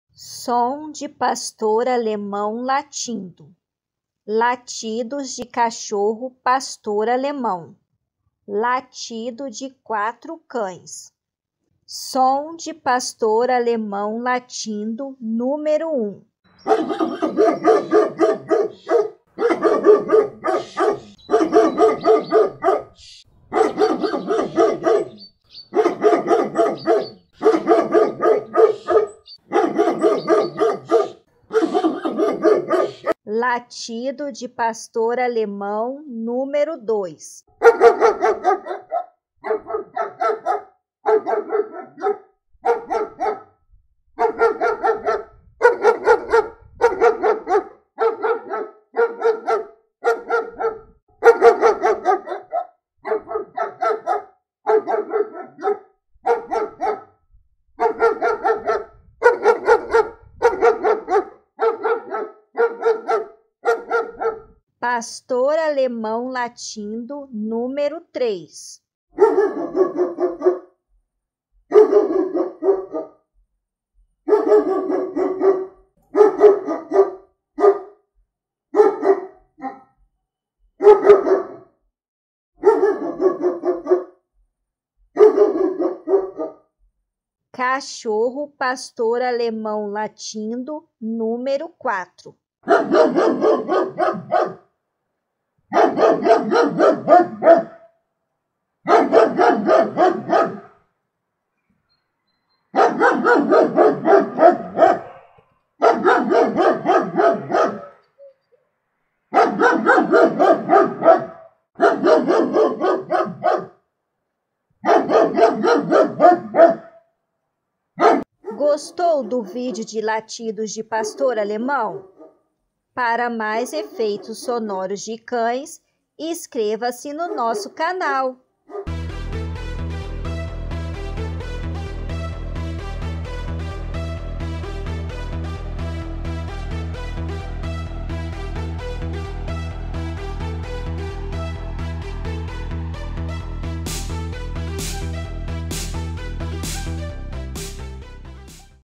Som de Pastor Alemão Latindo - Latidos de Cachorro Pastor Alemão [Latido de 4 Cães] 🐕‍🦺 🐶
Confira no vídeo som de pastor alemão latindo. Escolhemos latidos de 4 cães da raça pastor alemão para apreciar.

O pastor alemão também é conhecido como capa preta. É uma raça originária da Alemanha, daí o seu nome.

Pastor alemão em inglês = German Shepherd.

Utilizada como cão de pastoreio de rebanhos. Em pouco tempo ele provou que também é um cão policial, de guarda e companheiro.

Extremamente fiel ao seu dono. Não aceitam facilmente a presença de estranhos. Defensor de seu território.

Ele não late com frequência. Não é aquele tipo de cachorro que vive latindo sem parar. O pastor alemão late quando está brincando ou quando percebe que alguém invadiu a área que vive, e como um sinal de alerta, começa a latir para defender o seu território.

Não é recomendado ele ficar confinando em apartamentos, canis, lojas de pet shot ou casas pequenas.

Por essas razões é um cão adequado para ser criado em áreas maiores, como casas com quintais, sítios, chácaras, fazendas.

Além disso o capa preta precisa de um amplo espaço para se movimentar e gastar um pouco de sua energia. Assim evitará que fique estressado.

A expectativa de vida desta raça varia de 9 a 14 anos. Durante a Primeira Guerra Mundial foi utilizado amplamente como mensageiros, batedores, carregadores e sentinelas.

Muito utilizado pelos serviços policiais e no resgate de vítimas em ações de rastreamento, devido ao seu olfato apurado.

Raça de cão extremamente inteligente.

Gostaríamos que você mostrasse esse vídeo para o seu cachorro ou gato. Temos 4 sons de pastor alemão latindo, de forma forte, alta e até brava.

E veja se o seu animal reagiu ao barulho de cachorro latindo.

Conte para nós depois nos comentários!

Vamos adorar sua participação no canal!

🔴Aviso: Neste vídeo os sons emitidos não são exatamente as imagens respectivas do cachorro.